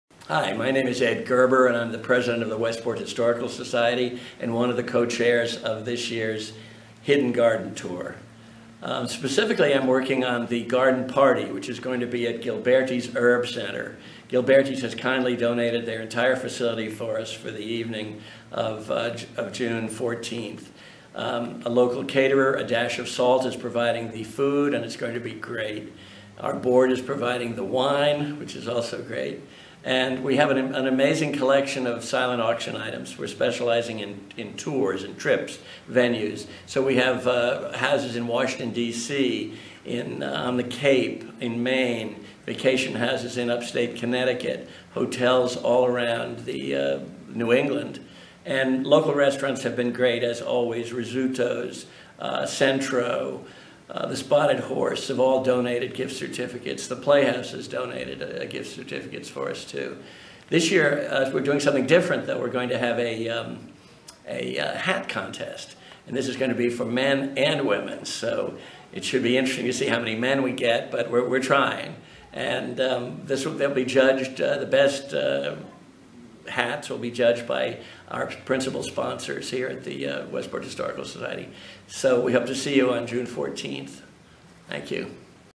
Ed Gerber, Westport Historical Society President Shares the Details on This Years Garden Party!
Ed Gerber, The Westport Historical Society's president shares all the details about this years garden party at Gilberties Herb Garden in Westport CT on June